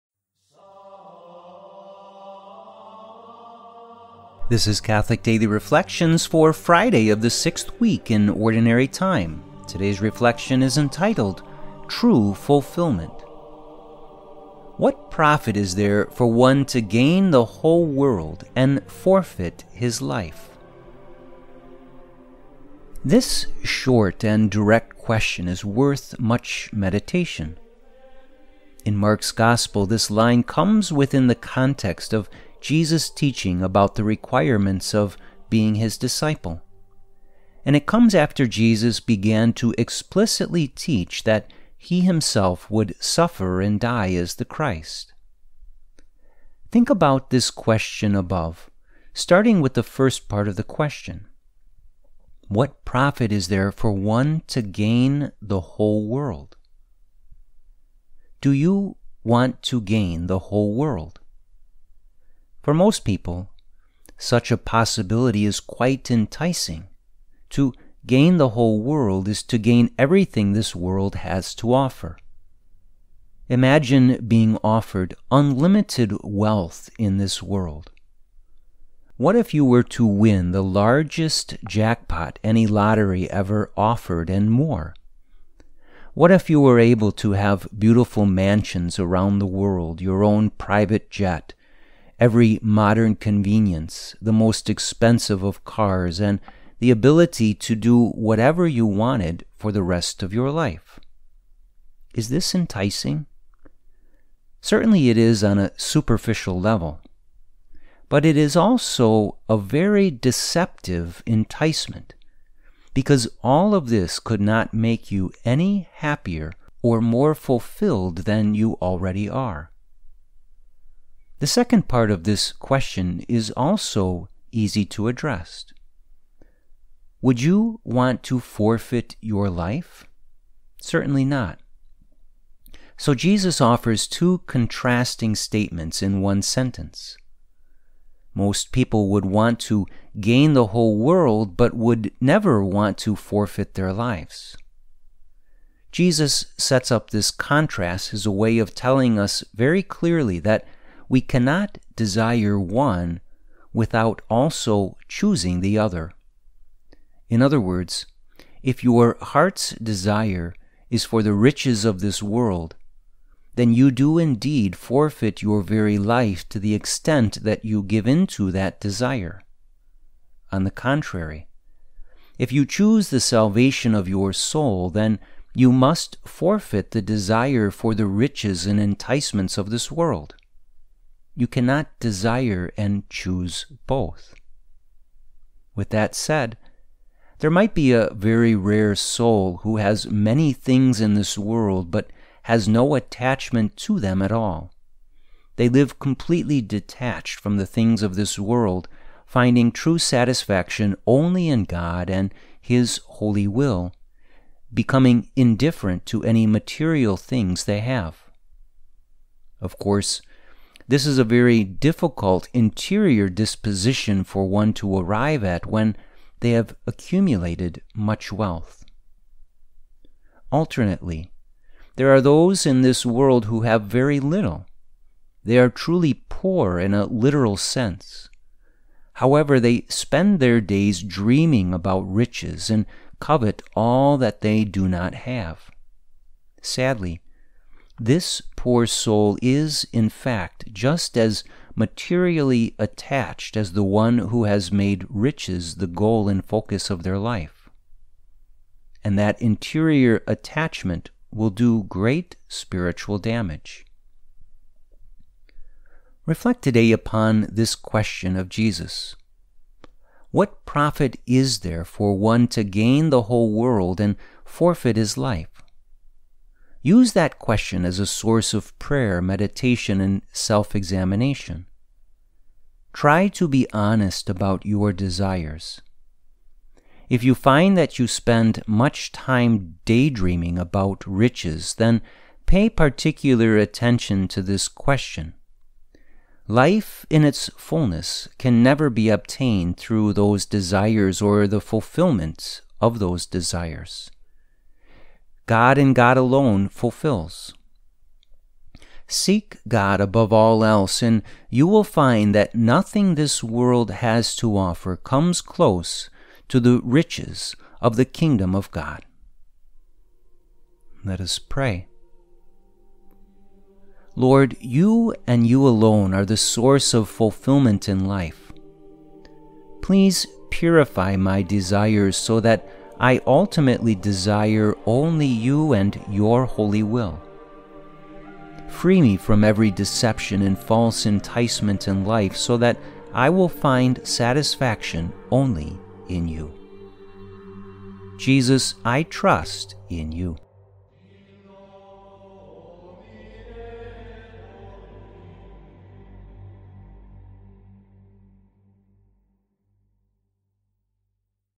True Fulfillment - Friday, February 17, 2023
“What profit is there for one to gain the whole world and forfeit his life?” Mark 8:36

This short and direct question is worth much meditation. In Mark’s Gospel, this line comes within the context of Jesus teaching about the requirements of being His disciple. And it comes after Jesus began to explicitly teach that He Himself would suffer and die as the Christ.

Think about this question above, starting with the first part of the question. “What profit is there for one to gain the whole world…” Do you want to gain the whole world? For most people, such a possibility is quite enticing. To “gain the whole world” is to gain everything this world has to offer. Imagine being offered unlimited wealth in this world. What if you were to win the largest jackpot any lottery ever offered and more? What if you were able to have beautiful mansions around the world, your own private jet, every modern convenience, the most expensive of cars, and the ability to do whatever you wanted for the rest of your life? Is this enticing?
Certainly it is on a superficial level. But it is also a very deceptive enticement, because all of this could not make you any happier or more fulfilled than you already are.

The second part of this question is also easy to address. Would you want to forfeit your life? Certainly not. So Jesus offers two contrasting statements in one sentence. Most people would want to gain the whole world but would never want to forfeit their lives. Jesus sets up this contrast as a way of telling us very clearly that we cannot desire one without also choosing the other. In other words, if your heart's desire is for the riches of this world, then you do indeed forfeit your very life to the extent that you give into that desire. On the contrary, if you choose the salvation of your soul, then you must forfeit the desire for the riches and enticements of this world. You cannot desire and choose both.

With that said, there might be a very rare soul who has many things in this world but has no attachment to them at all. They live completely detached from the things of this world, finding true satisfaction only in God and His holy will, becoming indifferent to any material things they have. Of course, this is a very difficult interior disposition for one to arrive at when they have accumulated much wealth.

Alternately, there are those in this world who have very little. They are truly poor in the literal sense. However, they spend their days dreaming about riches and covet all that they do not have. Sadly, this poor soul is, in fact, just as materially attached as the one who has made riches the goal and focus of life. And that interior attachment will do great spiritual damage.

Reflect, today, upon this question of Jesus: “What profit is there for one to gain the whole world and forfeit his life?” Use that question as a source of prayer, meditation and self-examination. Try to be honest about your desires. If you find that you spend much time daydreaming about riches, then pay particular attention to this question. Life in its fullness can never be obtained through those desires or the fulfillment of those desires. God and God alone fulfills. Seek God above all else and you will find that nothing this world has to offer comes close to the riches of the Kingdom of God.

Lord, You and You alone are the source of fulfillment in life. Please purify my desires so that I ultimately desire only You and Your holy will. Free me from every deception and false enticement in life so that I will find satisfaction only in You. Jesus, I trust in You.

CHAPTERS:
0:00 Beginning
0:14 Scripture Quote
0:23 Daily Reflection
4:18 Final Meditation
5:13 Closing Prayer